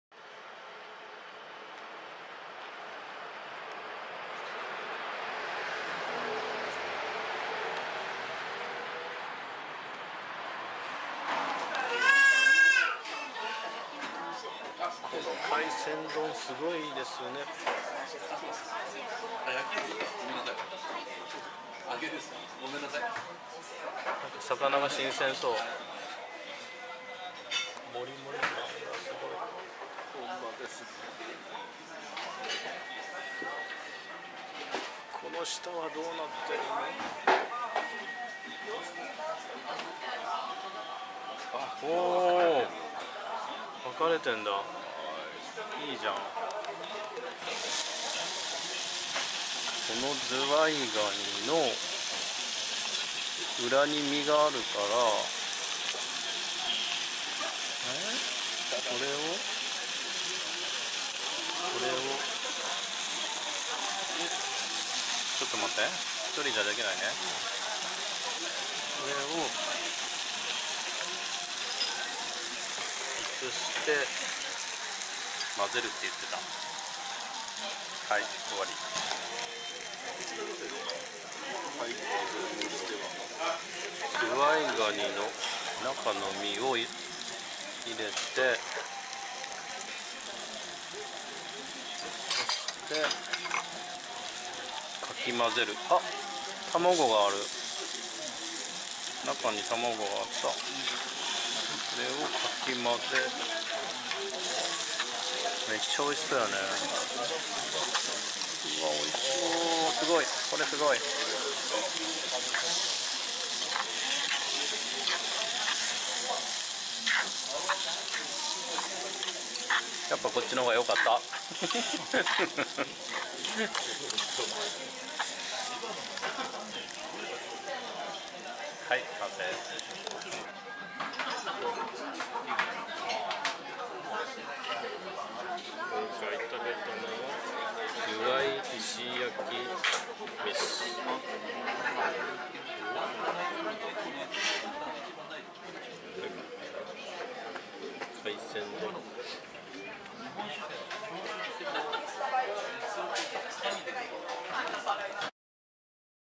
新潟直江津 軍ちゃん ずわい石焼飯VS海鮮丼
新潟県直江津の有名店　軍ちゃんでずわい石焼飯と海鮮丼を食べました。感動級のボリュームと美味しさでした。